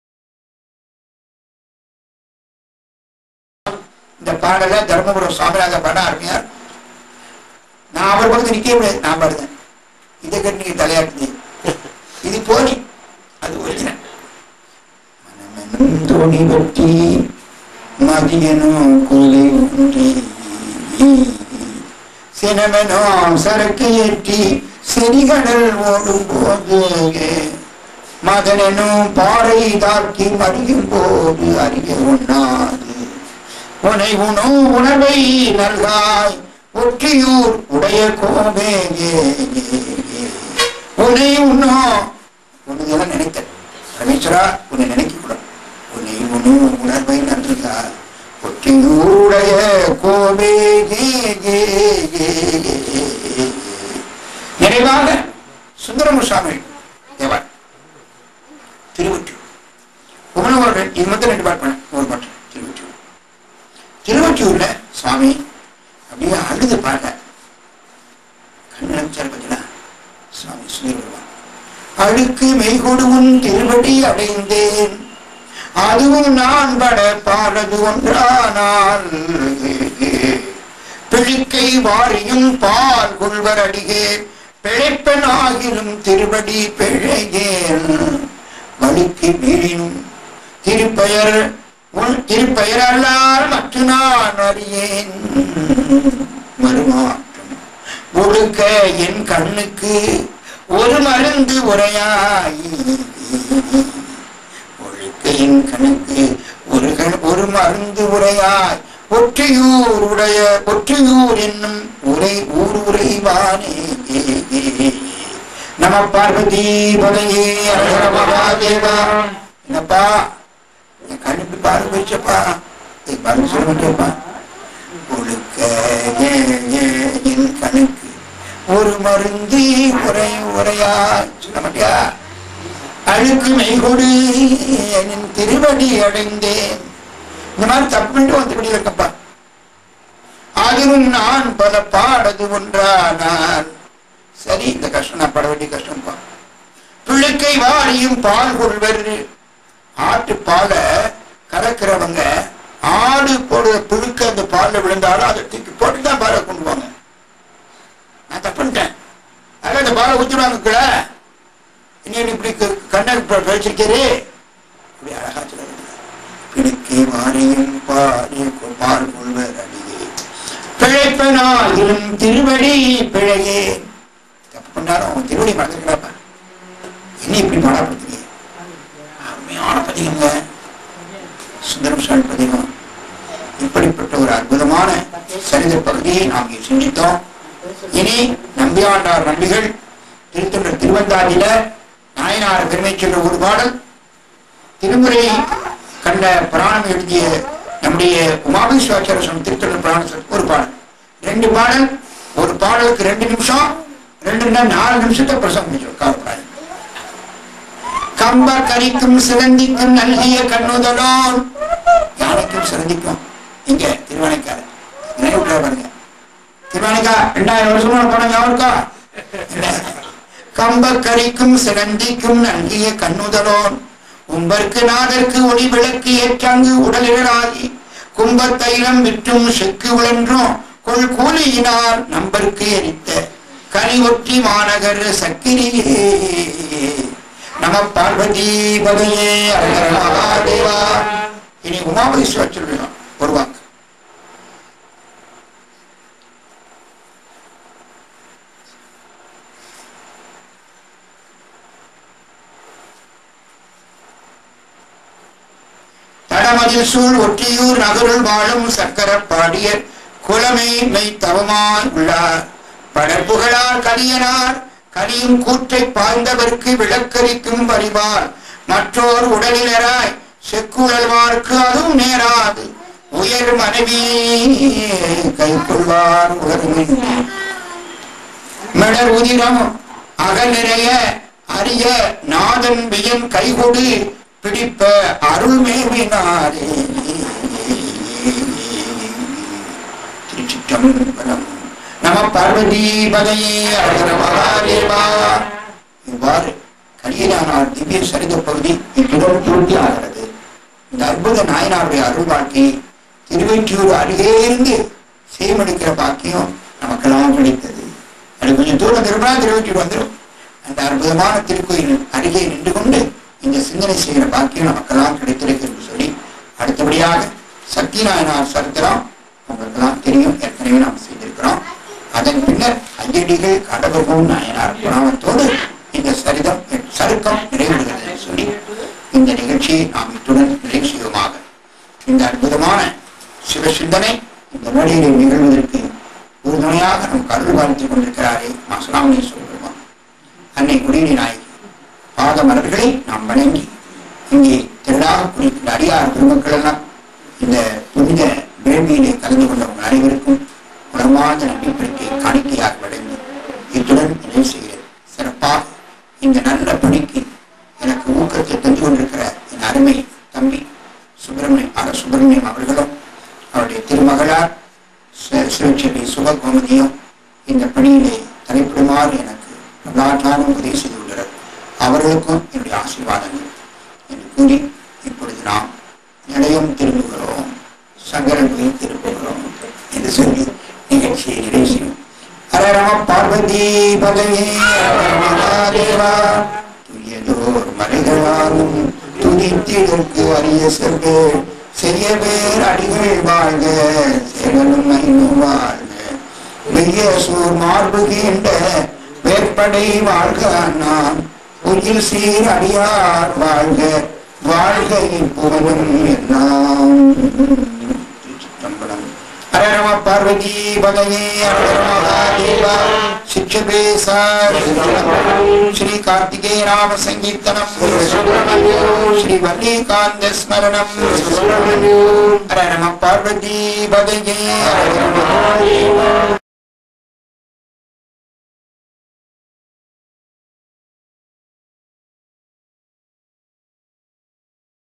கலிய நாயனார்-10 | Kaliya Naayanaar-10
பெரிய புராணம்.

சொற்பொழிவு. சைவ சரபம் மா.பட்டமுத்து

This video contains Speech on Life History of Kaliya Naayanaar by Saiva Sarabam M.Pattamuthu

ஏழாவது
பொய்யடிமையில்லாத புலவர் சருக்கம்

கலியநாயனார் புராணம்

தடமதில்சூ ழொற்றியூர் நகருள் வாழுஞ்
    சக்கரப்பா டியர்குலமெய்த தவமா யுள்ளார்
படர்புகழார் கலியனார் நலியுங் கூற்றைப்
    பாய்ந்தவர்க்கு விளக்கெரிக்கும் பரிவான் மற்றோ
ருடலிலராய்ச் செக்குழல்வார்க் கதுவு நேரா
    துயர்மனைவி யைக்கொள்வா ருளரு மின்றி
மிடறுதிர மகனிறைய வரிய நாதன்
    வியன்கைகொடு பிடிப்பவருண் மேவி னாரே.

    தொண்டைமண்டலத்திலே, திருவொற்றியூரிலேயுள்ள சக்கரப்பாடியிலே, செக்கார் குலத்திலே, கலியநாயனாரென்பவர் ஒருவர் இருந்தார்.  அவர் பெருஞ்செல்வராகி செல்வநிலையாமையையும் யாக்கை நிலைமையையும் உணர்ந்து, சிவபுண்ணியஞ் செய்தல்வேண்டும் எனத் தெளிந்து, அந்தத் திருப்பதியில் உள்ள சிவாலயத்தின் உள்ளும் புறம்பும் அல்லும் பகலும் எண்ணிறந்த திருவிளக்கு ஏற்றுவாராயினார்.

    பரமசிவன் நெடுங்காலம் இத்திருத்தொண்டைச் செய்துவரும் அவ்வடியாருடைய பத்திவலிமையைப் பிறர்க்குப் புலப்படுத்தும் பொருட்டு, அவரிடத்து உள்ள செல்வமெல்லாங் குன்றும்படி அருள்செய்தார்.  அவர் தாம் வறுமையெய்தியும் தமது மரபில் உள்ளோர் தரும் எண்ணெயை வாங்கிக் கூறிவிற்றுக் கொணர்ந்து, கூலி பெற்று, தாஞ்செய்யுந் திருத்தொண்டை வழுவாது செய்தார். சில நாளாயினபின் அவர்கள் கொடாதொழிய; அவர் மனந்தளர்ந்து, எண்ணெயாட்டும் இடத்திற்சென்று, தொழில் செய்து கூலி வாங்கி, திருவிளக்கிட்டார்.  பின்பு அத்தொழில் செய்வோர்கள் பலராய்ப் பெருகினமையால், அத்தொழிலால் வரும் பேறுங் கிடையாதுமுட்ட; ஒருநாள் கவலைகொண்டு, தம்முடைய மனைவியாரை விற்பதற்கு நகரெங்குங்கூறி, வாங்குவார் இன்மையால் மனந்தளர்ந்து, ஆலயத்தை அடைந்து, திருவிளக்கேற்றுஞ் சமயத்திலே, "திருவிளக்குப்பணி மாறில் நான் இறந்துவிடுவேன்" என்று துணிவுகொண்டு, திரியிட்ட அகல்களைப் பரப்பி, எண்ணெய்க்குப் பிரதியாகத் தமது இரத்தத்தை நிறைக்கும் பொருட்டு ஆயுதத்தினாலே கழுத்தை அரிந்தார்.  அப்பொழுது கிருபாசமுத்திரமாகிய பரமசிவன் நேர்வந்து அவருடைய கையைப் பிடித்து, அவருக்குமுன் இடபாரூடராய்த் தோன்றியருள; அவர் தாம் உற்ற ஊறுநீங்கி, சிரசின்மேல் அஞ்சலி செய்துகொண்டு நின்றார்.  சிவபெருமான் அவரைத் தமது திருவடியிலே சேர்த்தருளினார்.